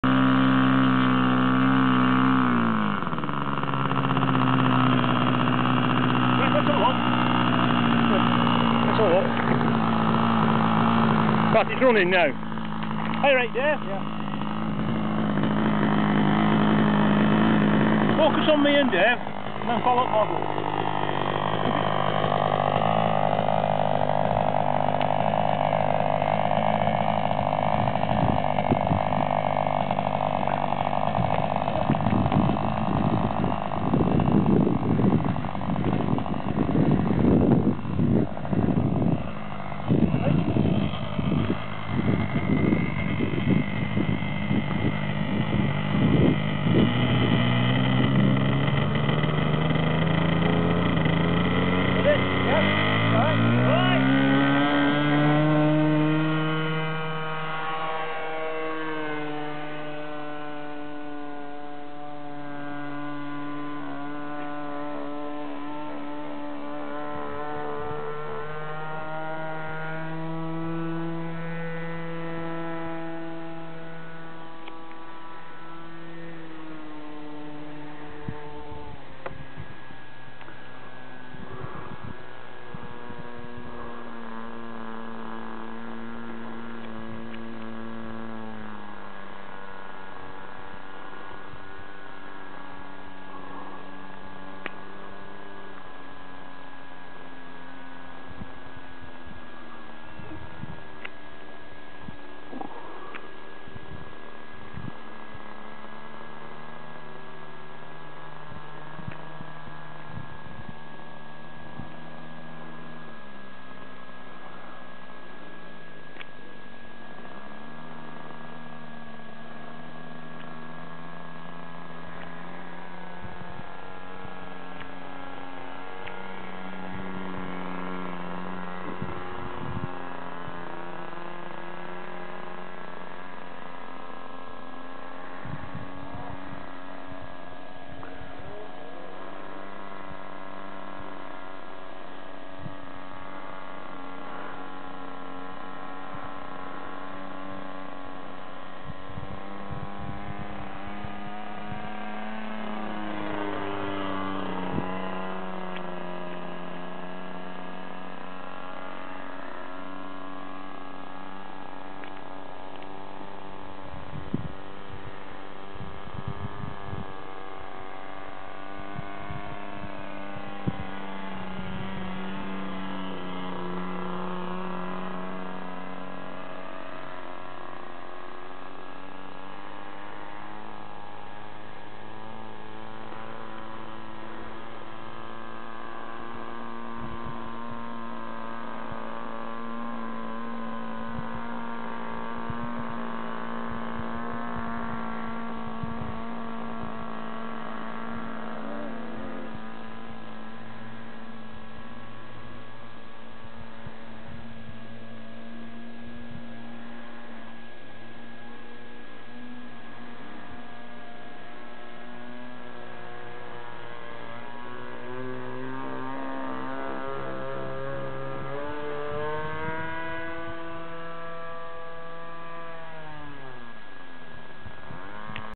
Sal Taibi Powerhouse - First Flight.
First flight of my Powerhouse on a nice, calm Lancashire evening.  Old, plain bearing OS 60 way past its peak and prime but more than enough power.
Also - Two old Duffers enjoying themselves but note the approaching storm clouds.